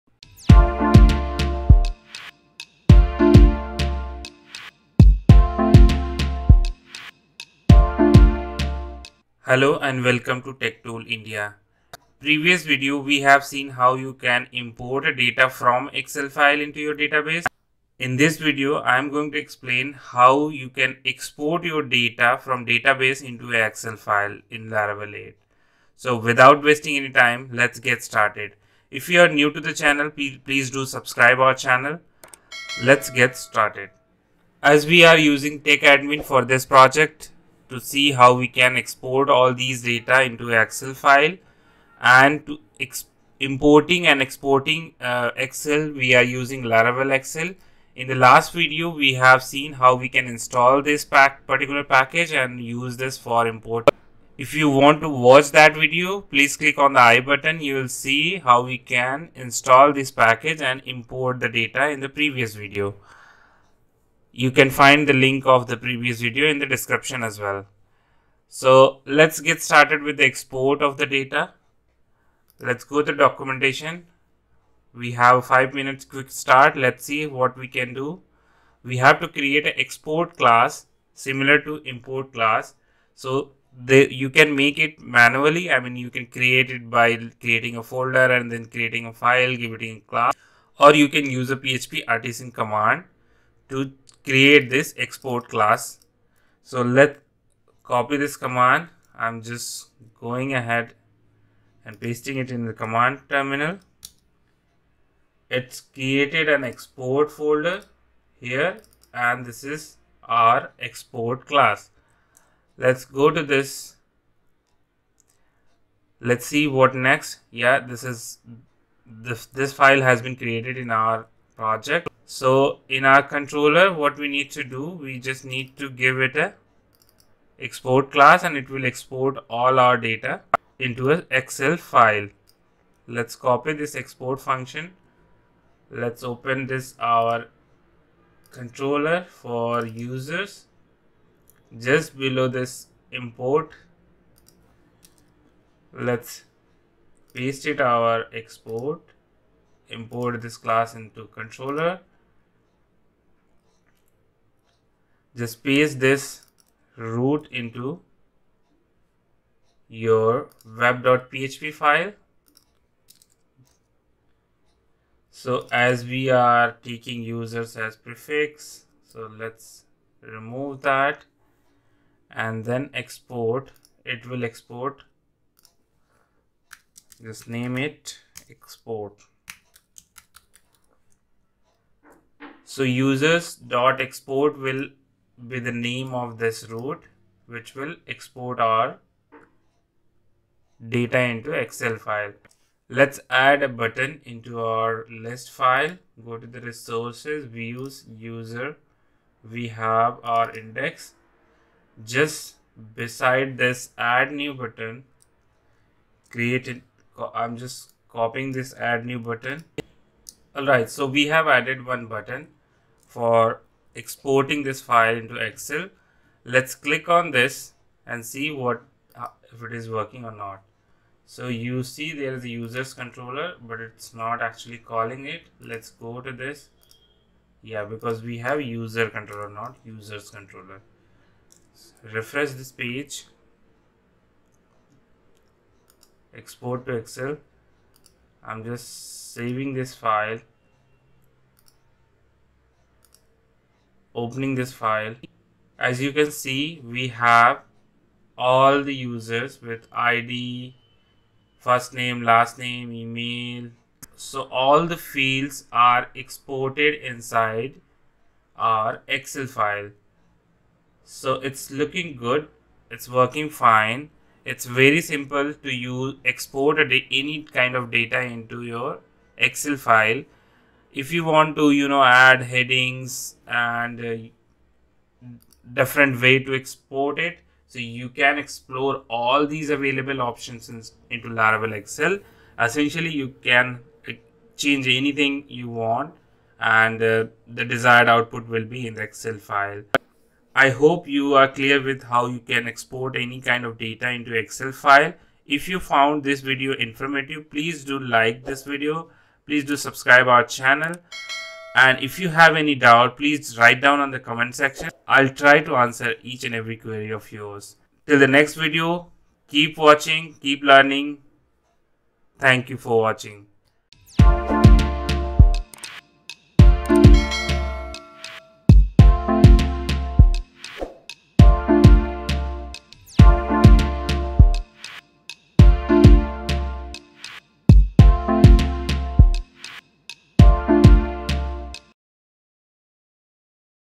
How to Export data to Excel in Laravel 8
In this video, I will provide an example of LARAVEL 8 export excel. you will learn Laravel 8 EXPORT to Excel. you can understand the concept of Exporting Excel files in LARAVEL 8. you will learn LARAVEL 8 Export file excel.

We will simply Export Users data to Excel File.

I have Used The Tech-Admin is a Free Laravel 8 Admin Starter Kit you can access the GitHub link:

For any kind of queries or custom development do comments on the video, we will try to solve every problem of yours.